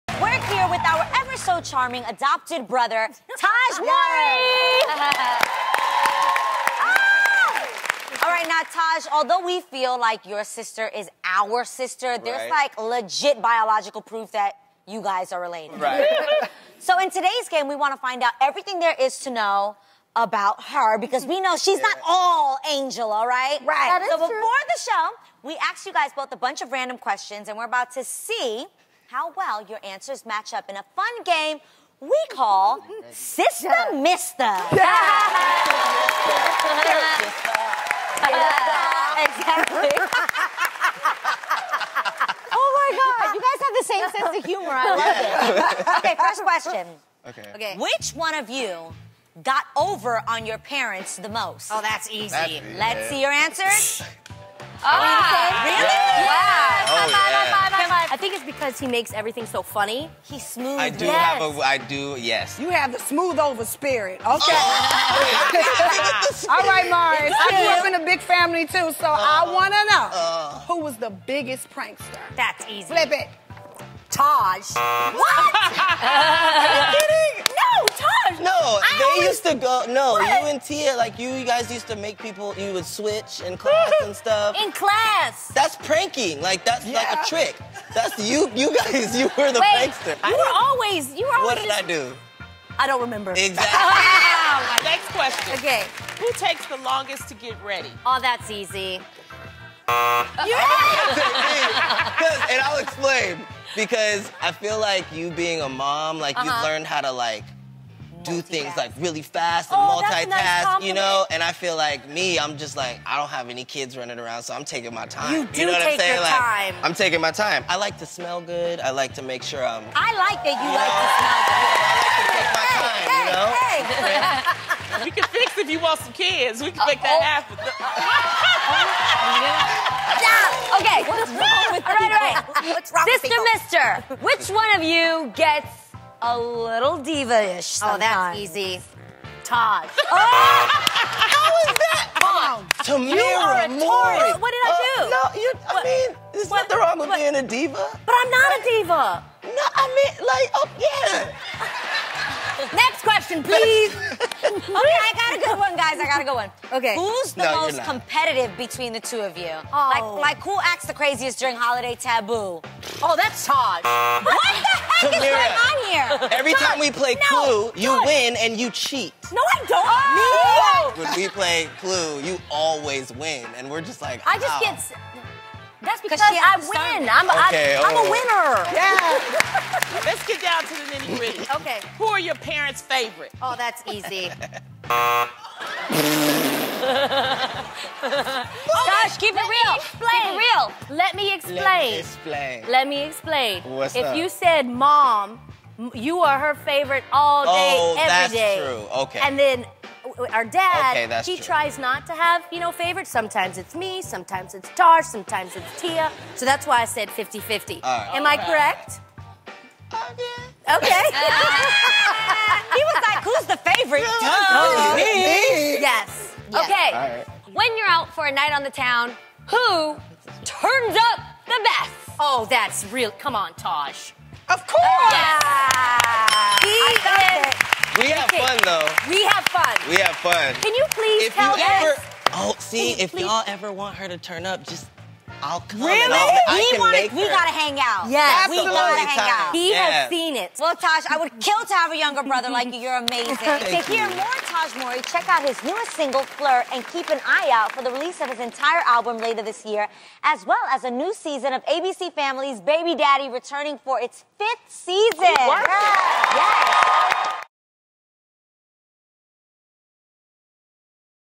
Let’s Play Sister Mister
In a mission to find out everything there is to know about Tamera, the hosts put her little brother Tahj to the test in a fun game of Sister Mister.